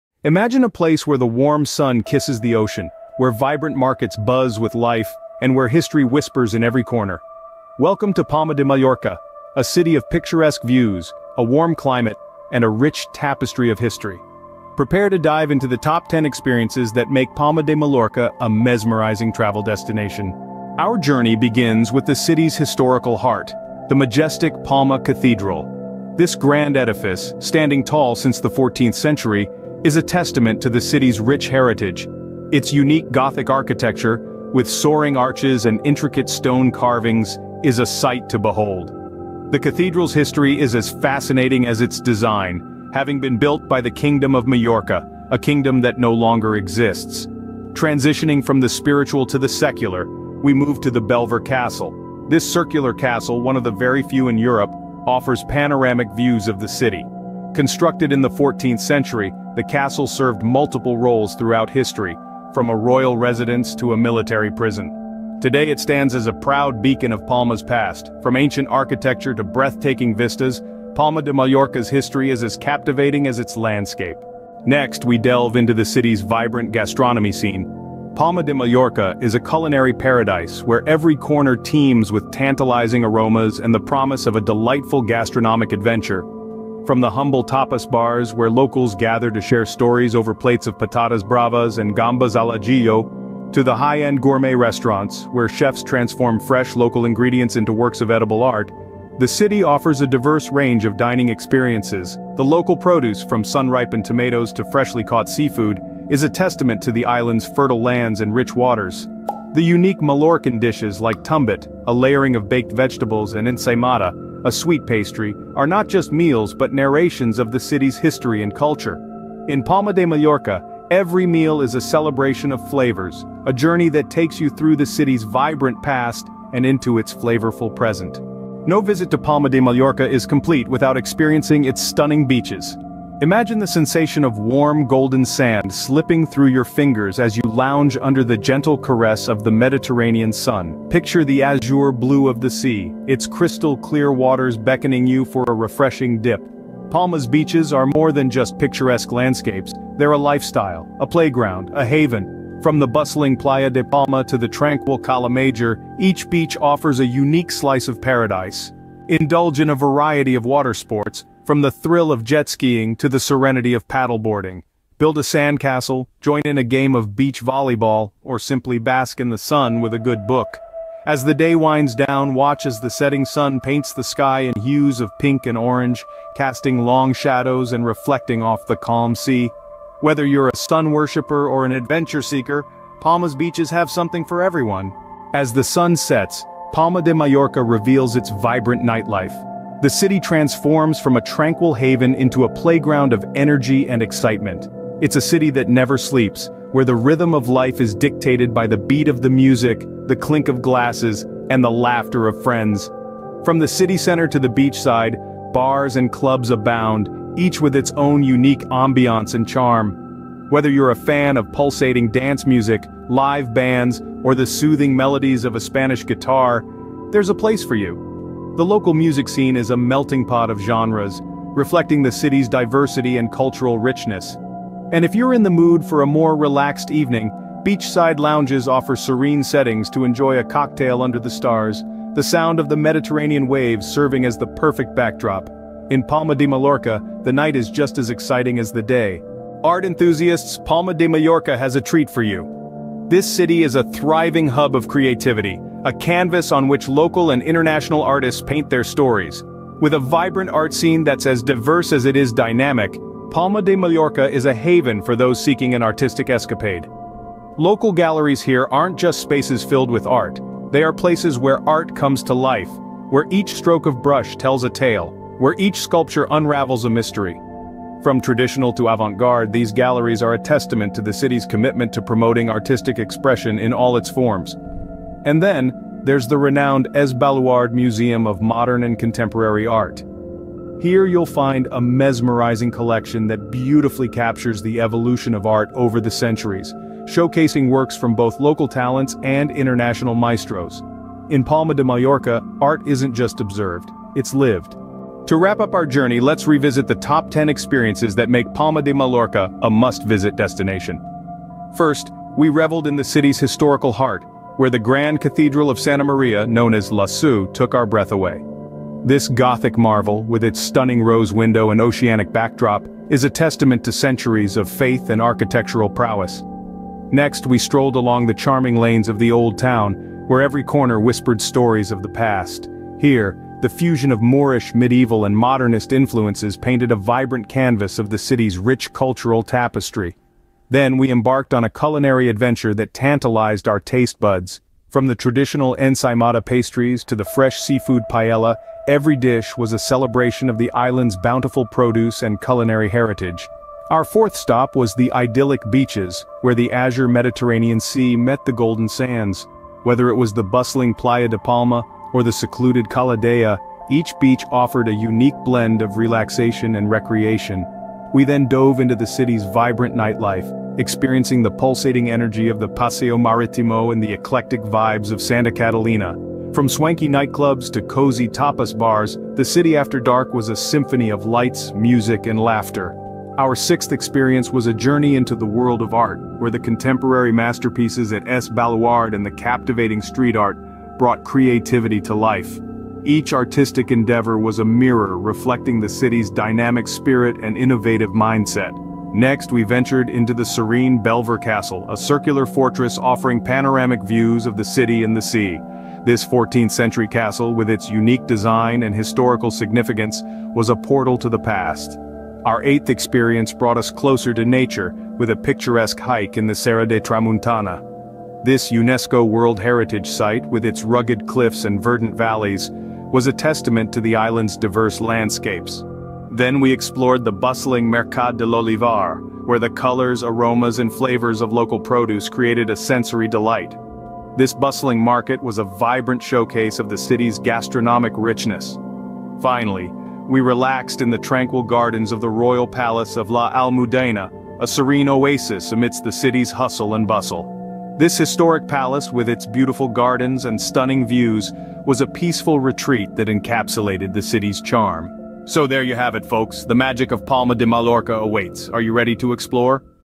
Mallorca Top 10 Things To Do Travel Guide
🎥 Join us on a journey through Mallorca Top 10 Things To Do 🏝️

Embark with us on an exploration of the unforgettable experiences this vibrant island has to offer! From iconic landmarks like Palma Cathedral to hidden gems like the idyllic coves of the east coast, we've ventured everywhere. 🌟 Whether you're a local or a first-time visitor, don't miss out on the opportunity! 🇪🇸✨

00:00:00 Unveiling Palma de Mallorca
00:00:20 A Stroll through History
00:01:14 The Culinary Delights
00:02:08 The Beach Life
00:03:05 The Vibrant Nightlife
00:03:59 The Artistic Flair
00:04:54 The Grand Finale